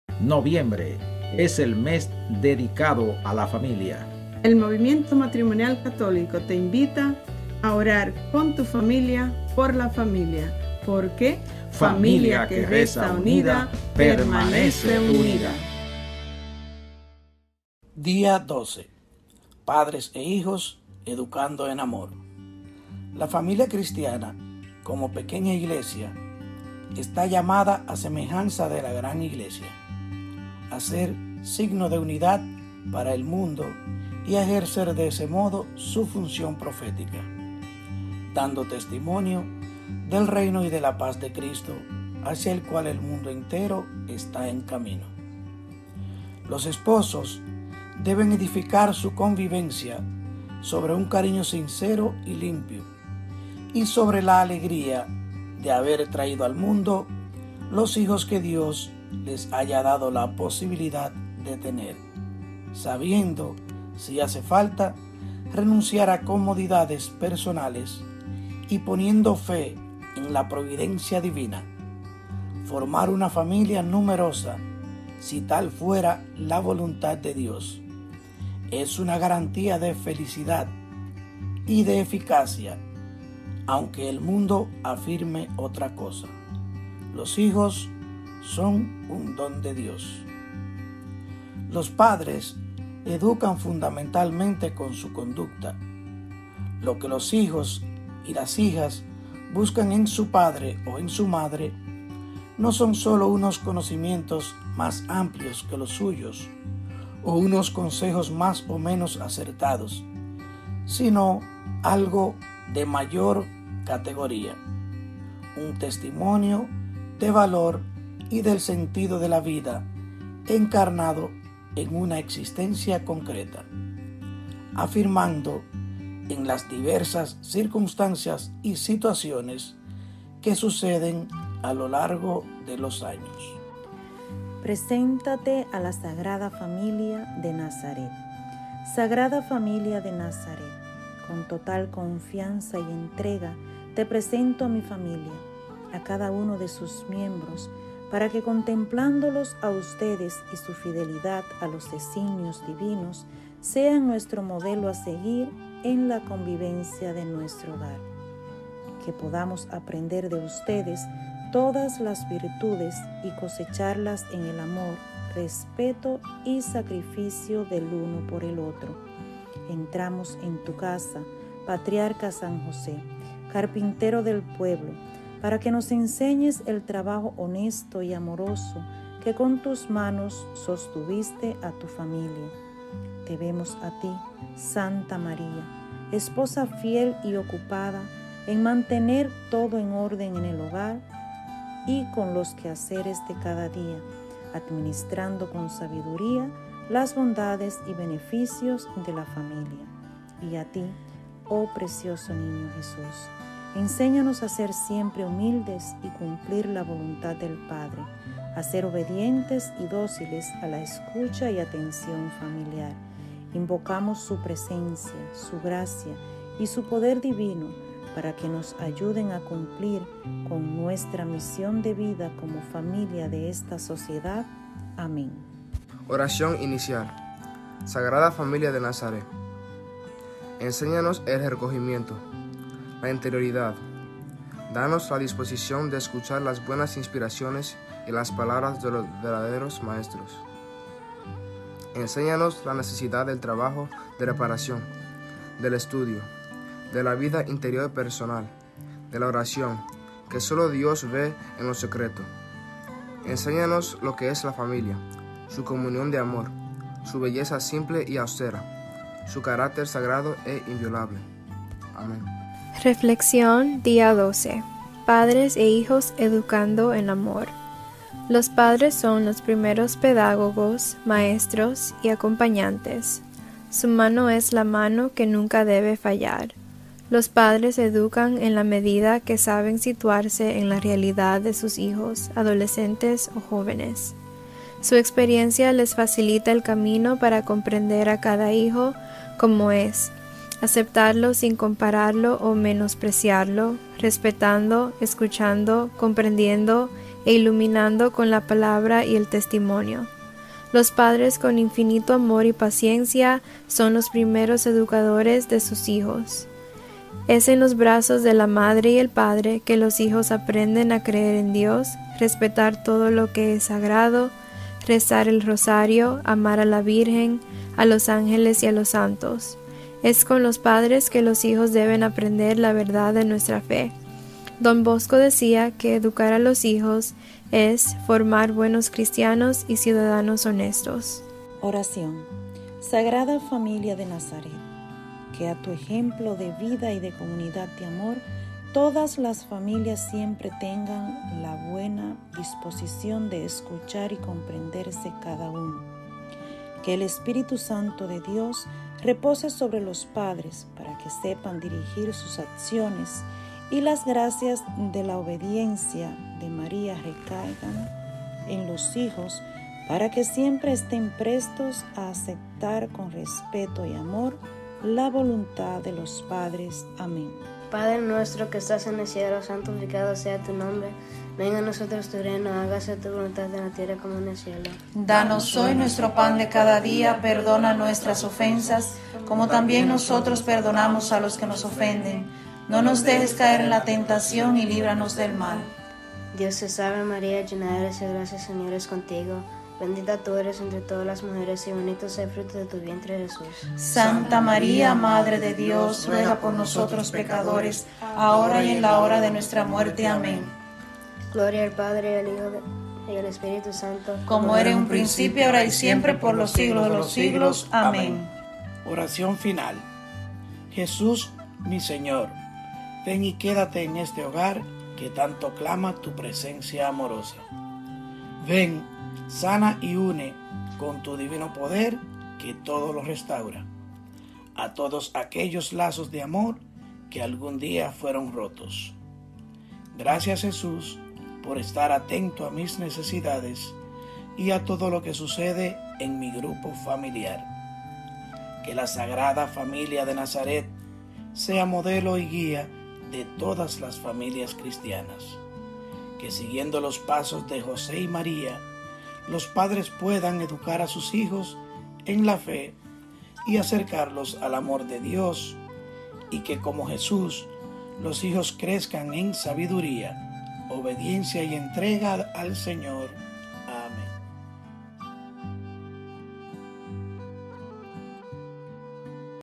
MMC ORANDO EN FAMILIA DIA 12
Te inivitamos a que por medio de estas reflexiones nos acompañen  a meditar durante el mes de noviembre  dedicado a la familia y el matrimonio.

Dejemos que la sagrada familia de nazareth sea nuestro modelo de oración y derrame sus bendiciones sobre nosotros. Amén.

123 CREEMOS EN EL MATRIMONIO.

MUSIC